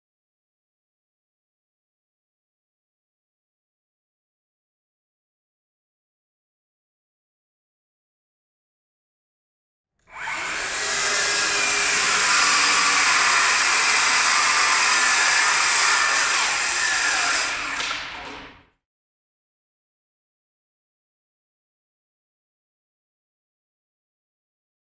DEWALT Power Detect Circular Saw Comparison - DEWALT DCS574 / DEWALT DCS570
Trying to decide which circular saw is best for your job or project? See how the increased power of the DEWALT® Power Detect™ Circular Saw performs against the XR® Circular Saw in this quick head-to-head demo.